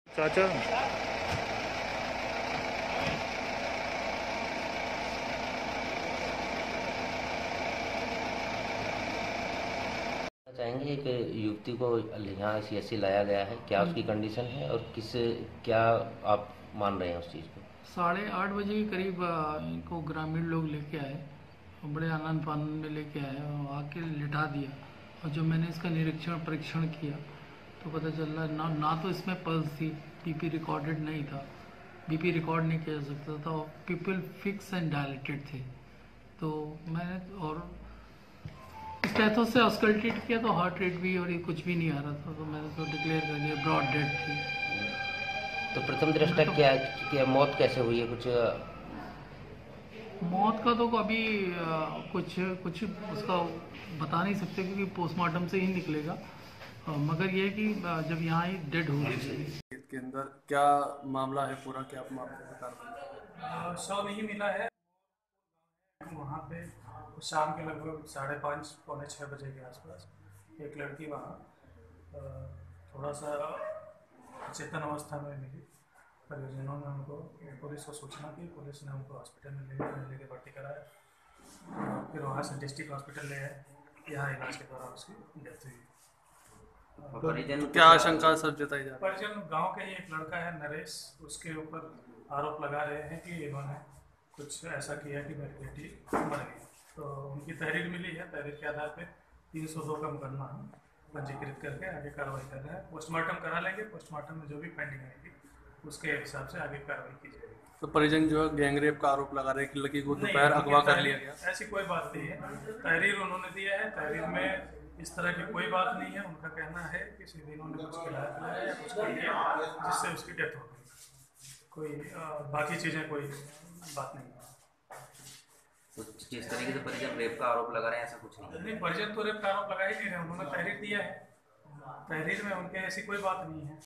हापुड़ में युवती की गैंगरेप के बाद हत्या
हापुड़ : यूपी में एक बार फिर हैवानों ने एक युवती को अपनी हवस का शिकार बनाया है. मामला दिल्ली से करीब 50 किलोमीटर दूर बसे जनपद हापुड़ के थाना हाफिजपुर का है. पीड़ित परिजनों का आरोप है कि तीन हैवानों ने युवती को अगवा कर नशीला पदार्थ खिलाकर जंगल में ले गए फिर उसके साथ गैंगरेप किया.